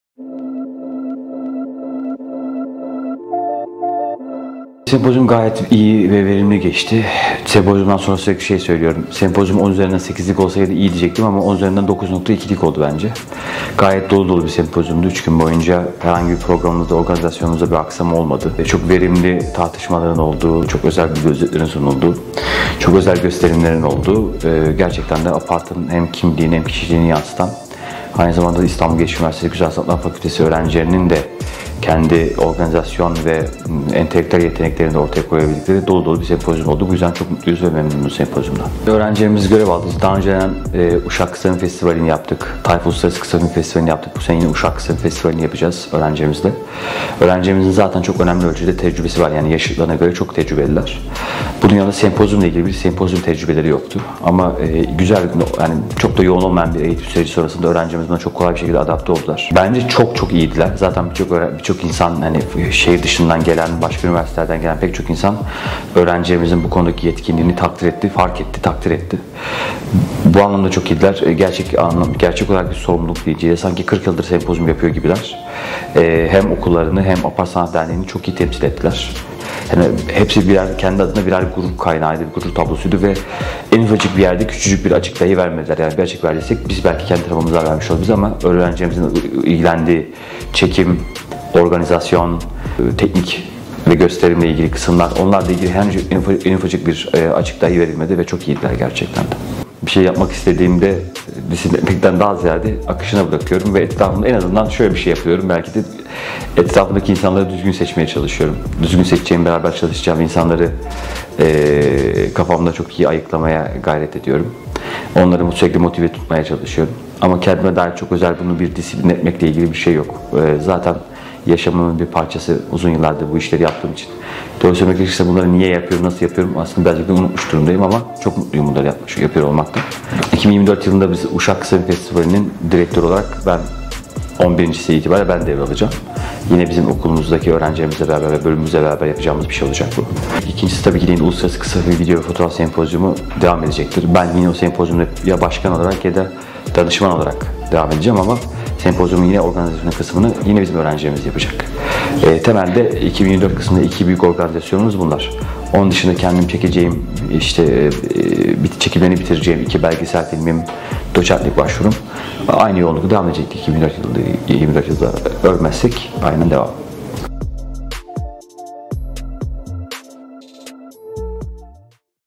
Dr. Öğr. Üyesi Emre Doğan'la Sempozyumu ve Yeni Dönem Planlarını Konuştuk!
İstanbul Gelişim Üniversitesi (İGÜ), Güzel Sanatlar Fakültesi (GSF), Radyo, Televizyon ve Sinema Bölüm Başkanı Dr. Öğr. Üyesi Emre Doğan ile yürütücülerinden olduğu “Uluslararası Kısa Film, Video ve Fotoğraf Sempozyumu” ve gelecek projeleri hakkında röportaj gerçekleştirildi.